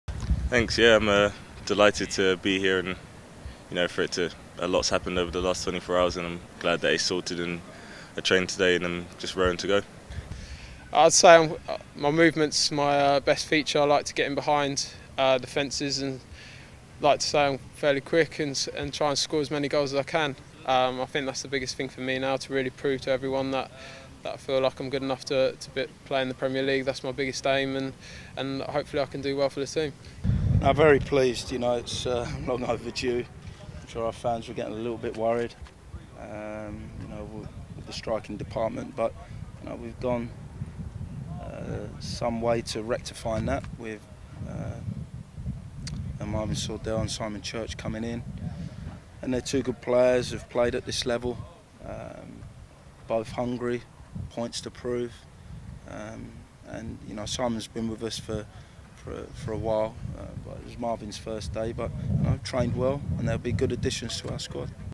What's on CAFC Player today - Charlton Athletic
Watch video interviews with Marvin Sordell, Simon Church and Chris Powell on CAFC Player now!